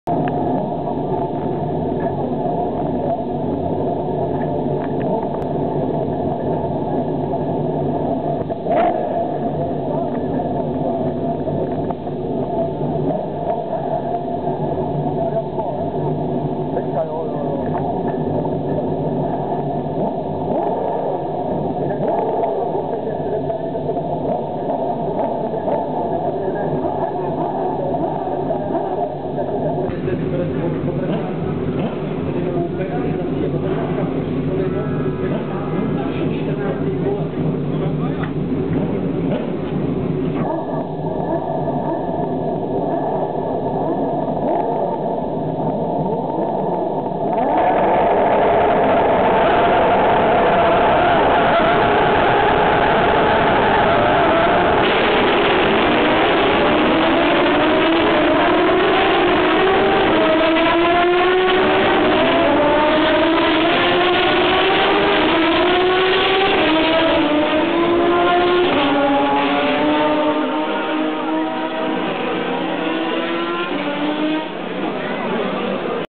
Warm up lap start, Formula Renault 3.5 Series, 2010 Brno World Series by Renault
Start do zahřívacího kola, Formula Renault 3.5 Series, 2. závod 2010 Brno World Series by Renault

Warm up lap start, Formula Renault 3.5 Series, 2nd race, 2010 Brno World Series by Renault